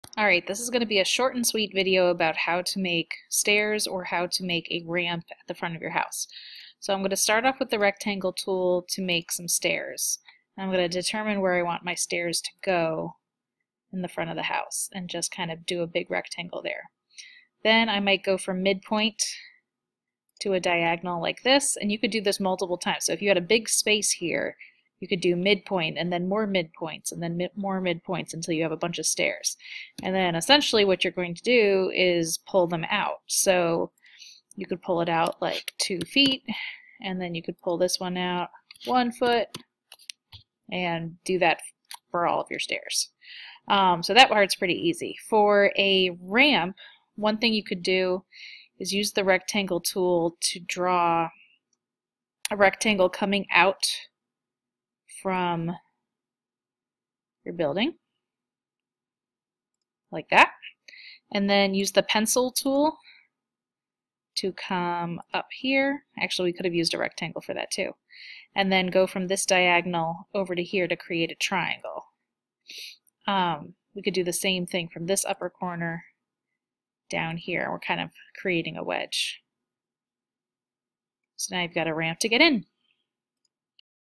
Sketch-Up - Porch Steps and Ramp
How to make front steps or a ramp in the free online SketchUp.